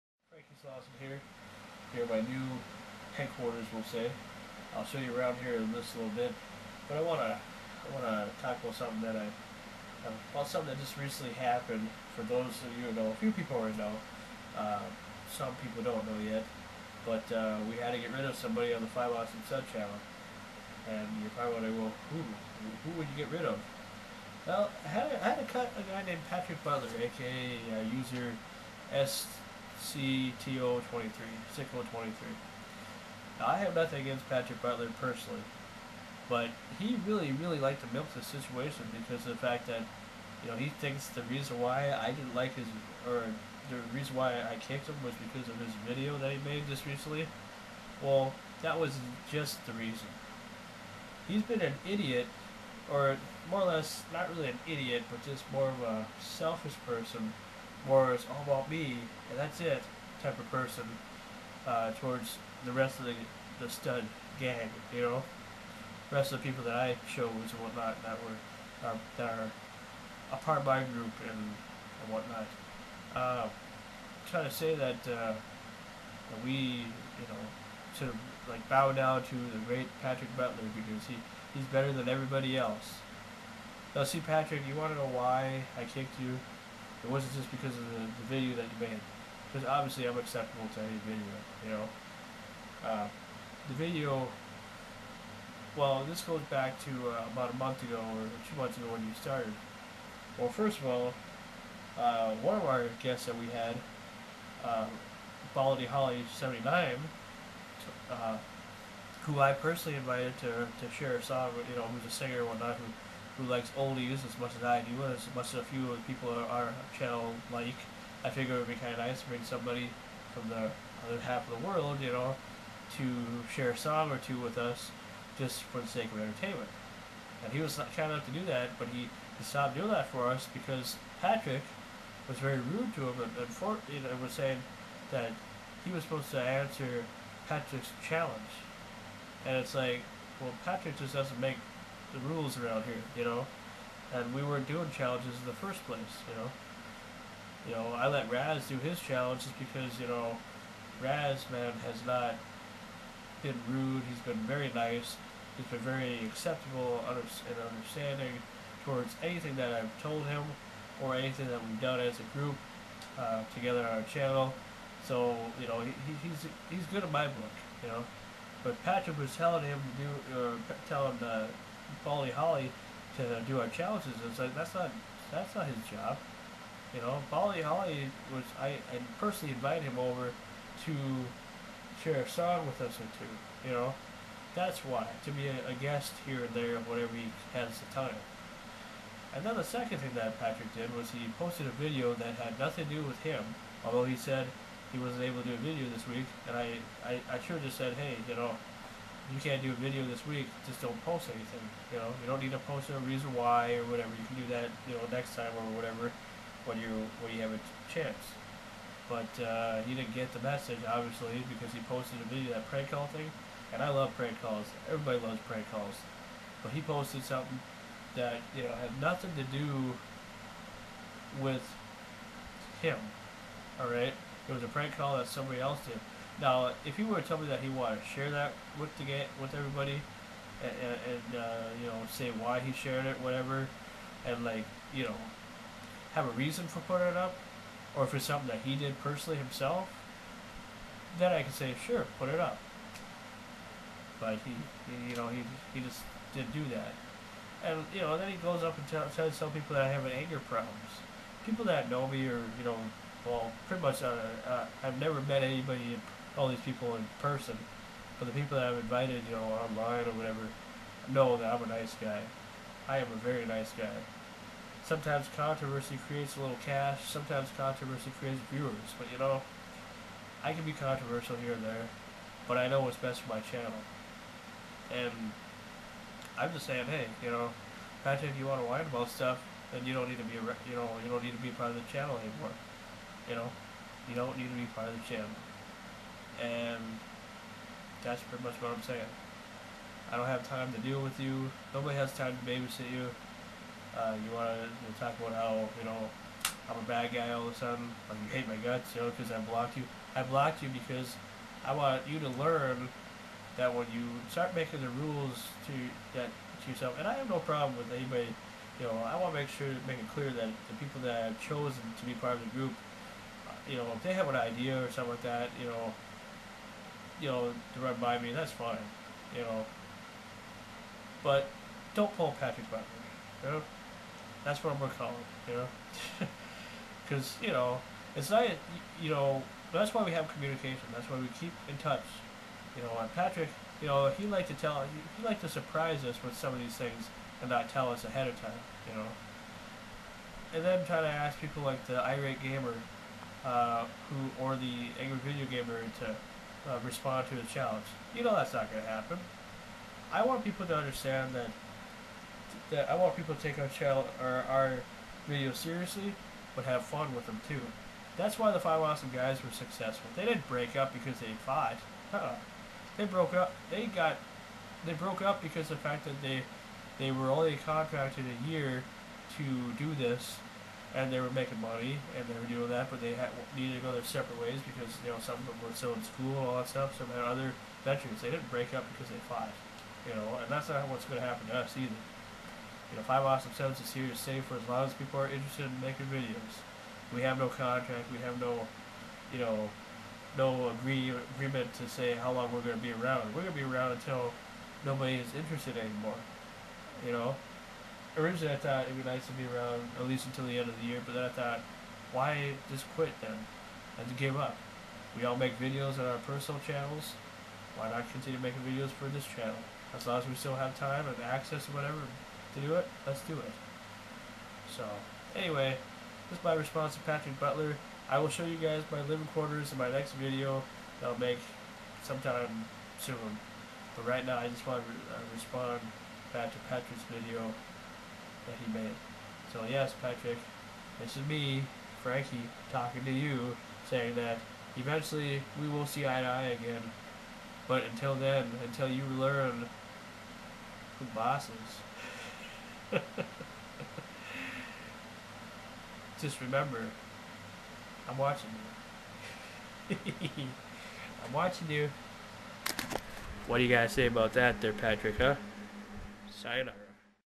Patrick My Boy someday............you'll understand
OK I'm gonna be as nice as I can to explain the REAL Truth Behind this whole Patrick Butler Situation, feel free to give some comments below on what I should do with this ordeal! Thanks Rock On People!